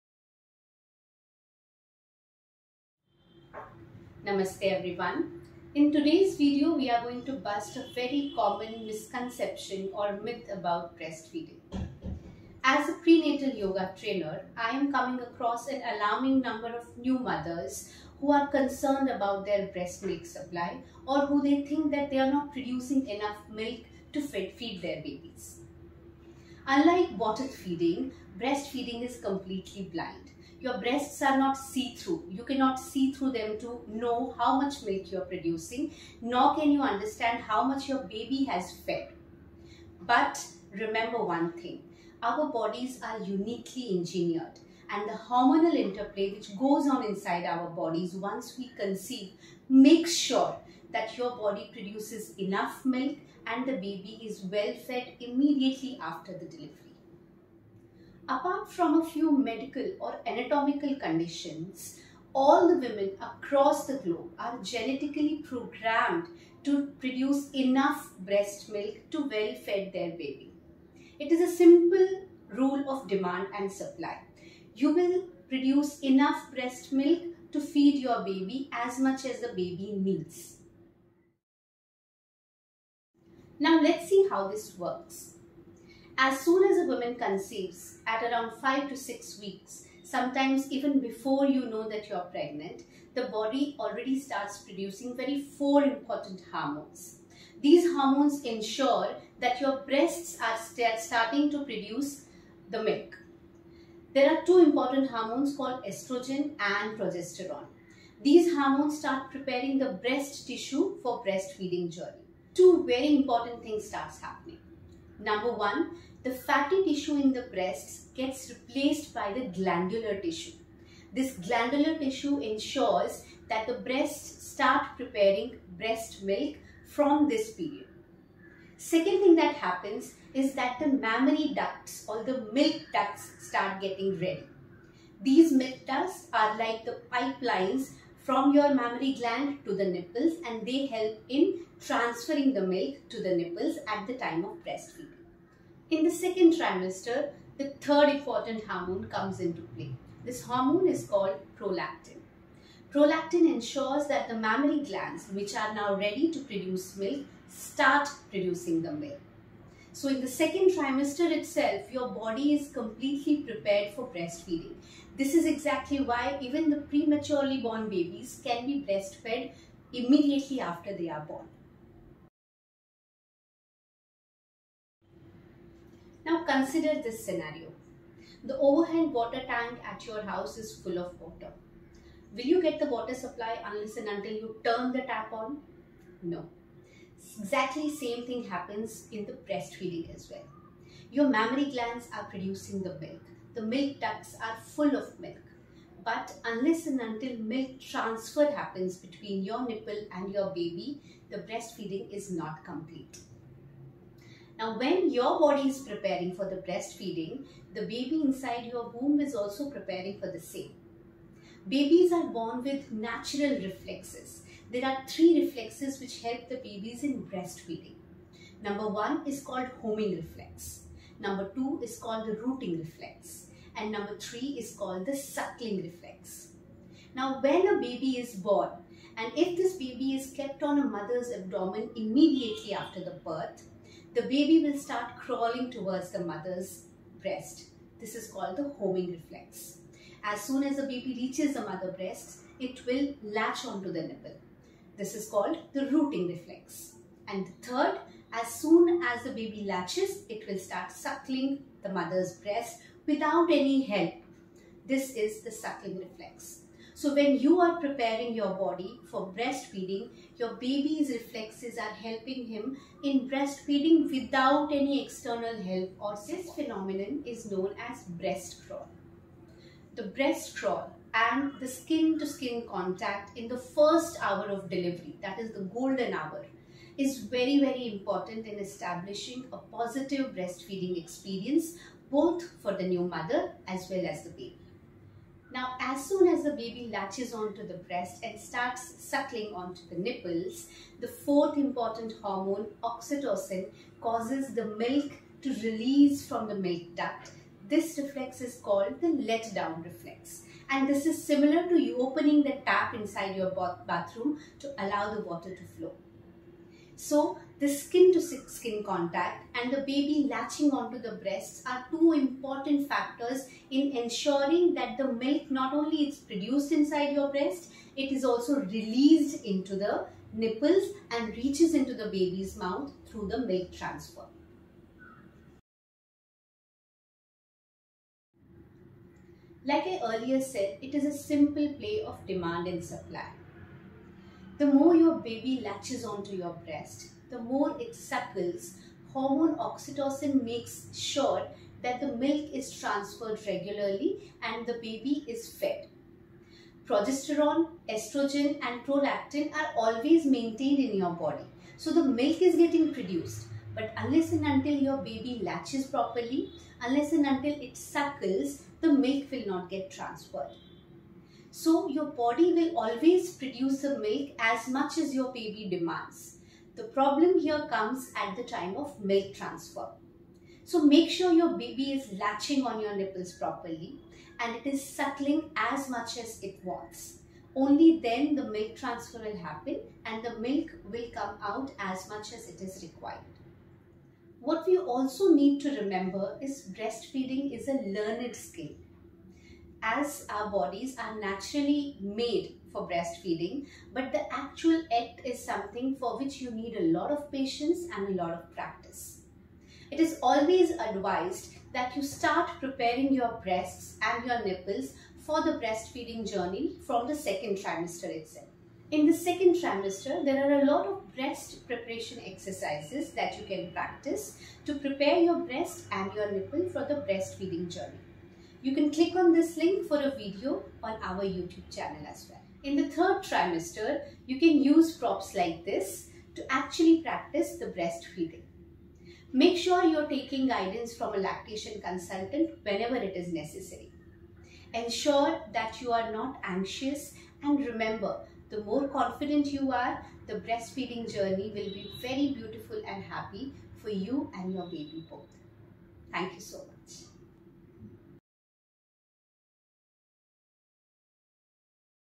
Understanding Breastfeeding - The science of milk supply
In this enlightening video, we explore the extraordinary process that unfolds within a female body during the pregnancy & after delivery, ensuring the creation of the most exquisite and nourishing elixir for her newborn, the breastmilk.

Join us as we demystify the science behind this natural wonder, exploring the intricacies of hormone regulation, gland development, and the fascinating interplay of biological signals that orchestrate the production of adequate breast milk.